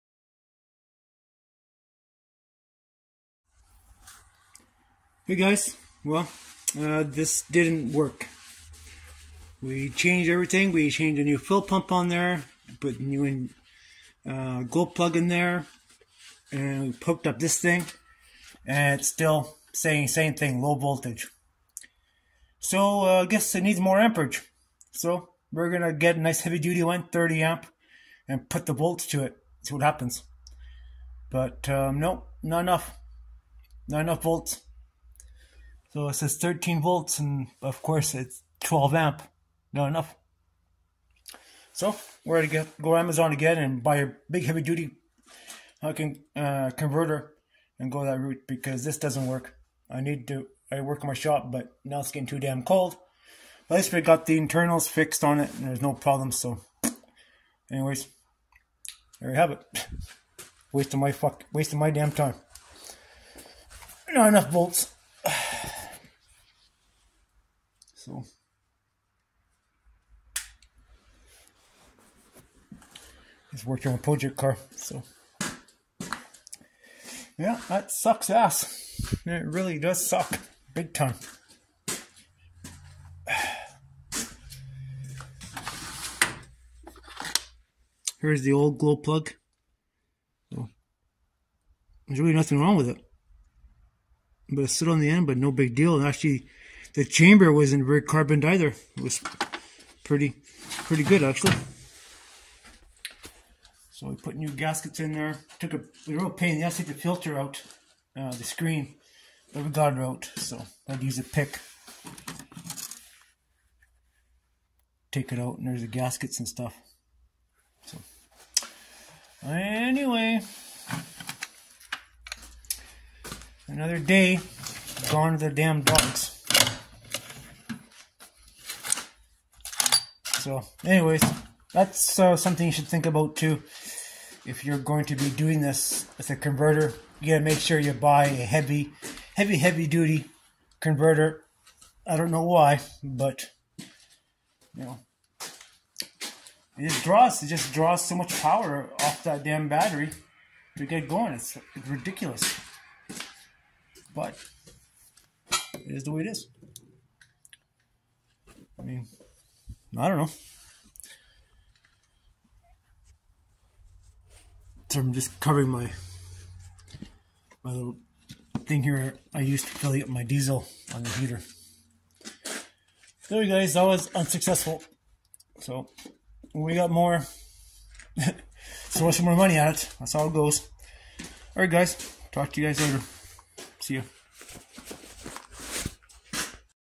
New Parts Installed On Diesel Heater -- But No Start PT 3
So I installed new parts well just the glow plug and new screen and connected converter and still no start. The heater is sending a E01 CODE low power . More testing coming up .